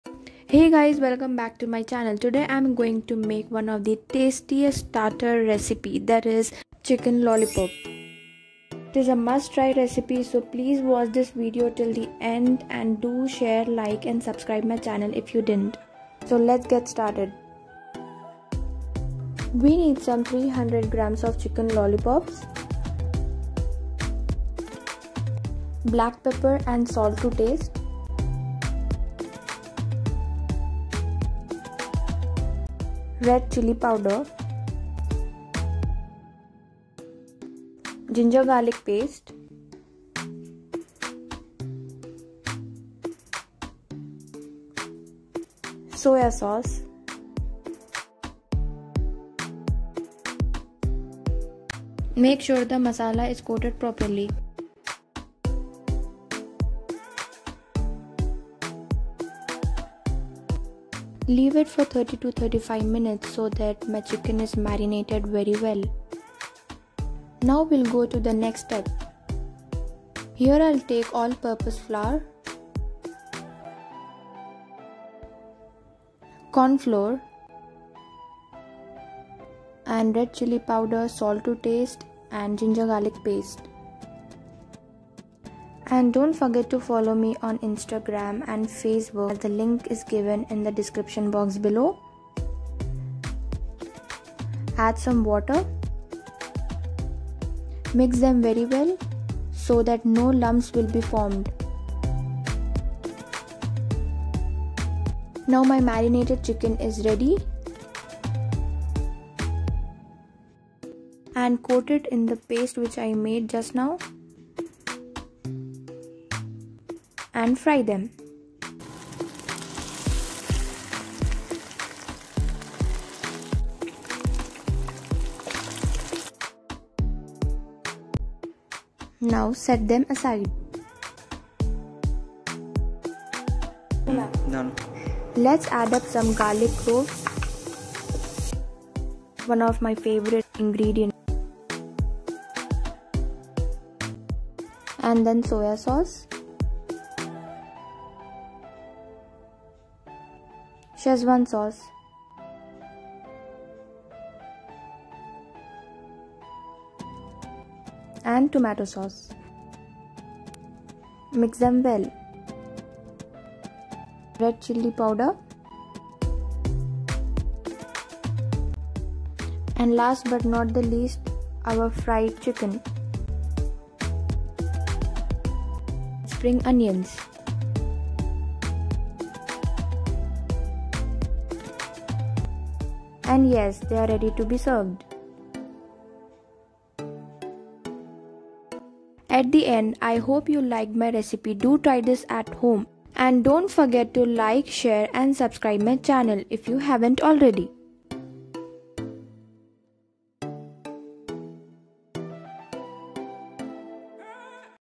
CHICKEN LOLLIPOP RECIPE | Indo-Chinese chicken starter|| Easy to make at home ~ Mehzari's kitchen
Do try it and share your reviews in the comment section box below.

INGREDIENTS:

Chicken lollipop-300 gm
All purpose flour(Maida)-50 gm
Corn flour-25 gm
Black pepper powder(kali mirch)-1/2 tsp
Ginger garlic paste-1 tbsp
Red chilli powder(kashmiri)-1.5 tsp
Soy sauce-3 tsp
Schezwan sauce-1 tbsp
Tomato sauce-1.5 tbsp
Finely chopped garlic-8 to 10 cloves
Chopped spring onion
Cooking oil-250 ml